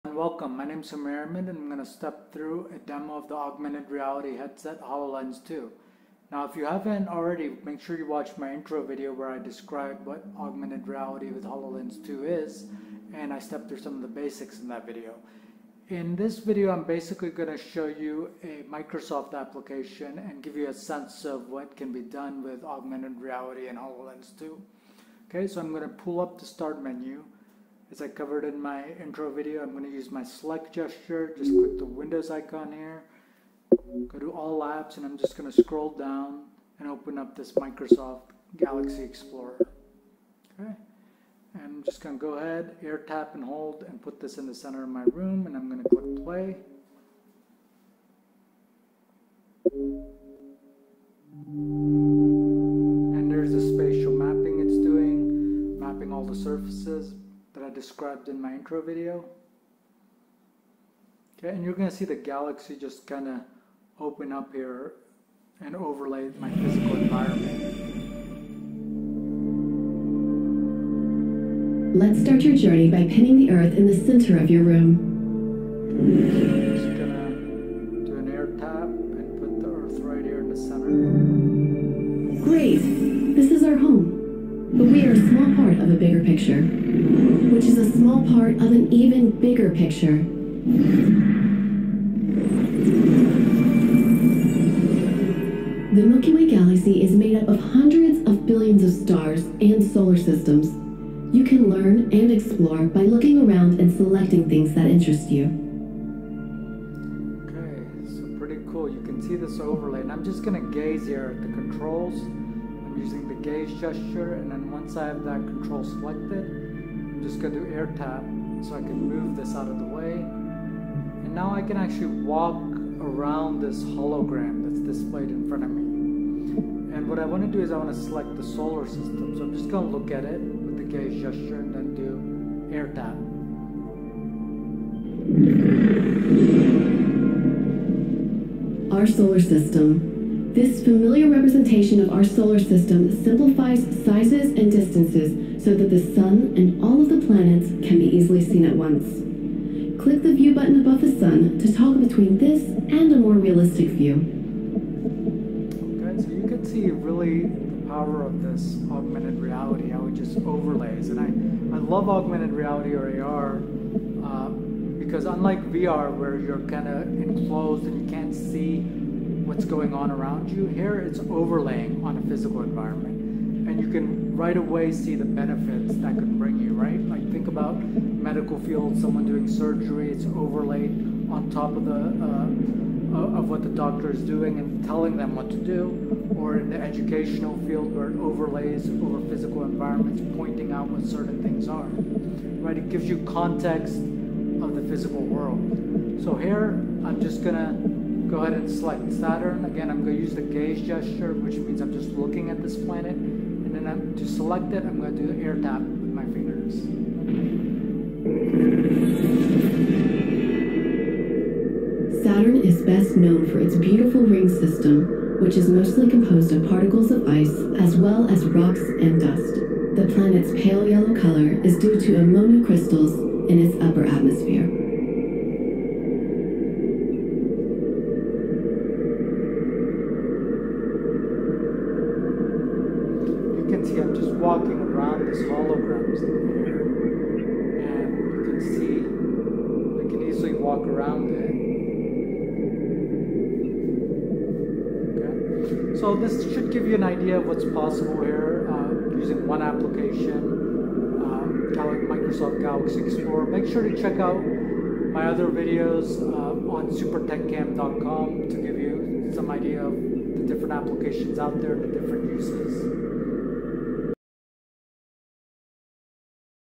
Augmented Reality with Hololens 2 - Galaxy Explorer
Short video highlighting the augmented reality capabilities of Microsoft Hololens 2 by demonstrating the Microsoft Galaxy Explorer App.